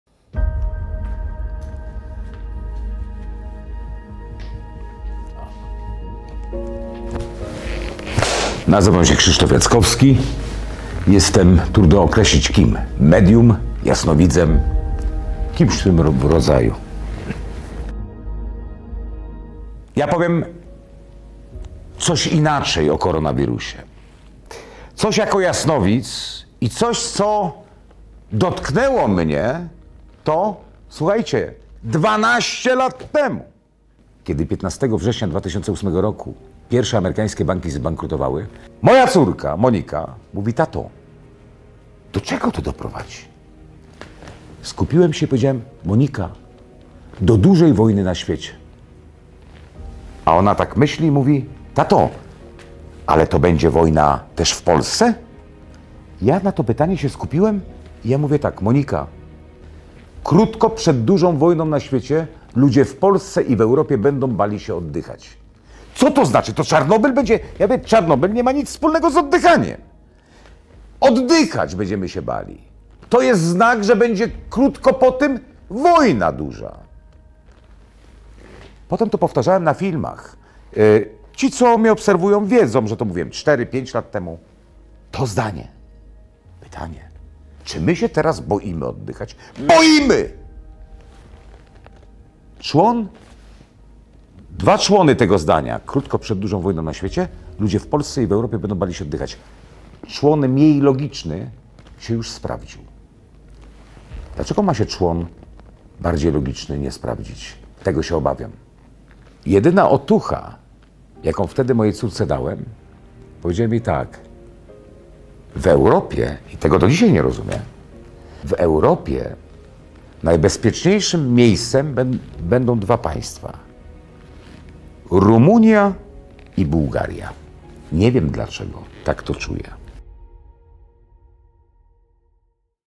Jackowski przewidział pandemię 12 lat temu: „To wstęp do wojny…”
O swojej niezwykłej wizji 57-latek z Człuchowa opowiedział przed kamerą VIVY.pl. Krzysztof Jackowski powiedział swojej córce o koronawirusie już w 2008 roku. Co więcej wtedy zobaczył? | Więcej